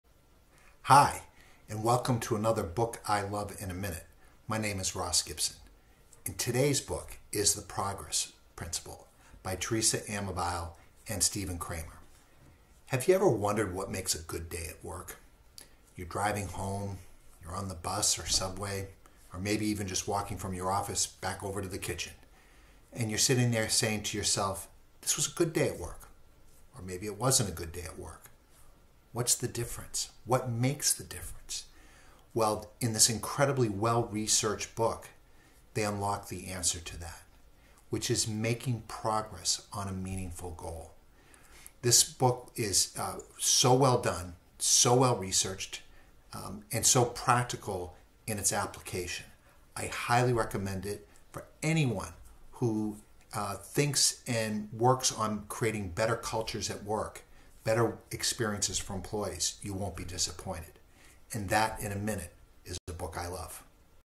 A book I love: The Progress Principle by Teresa Amabile and Steven Kramer
A one minute review of a book I love, The Progress Principle by Teresa Amabile and Steven Kramer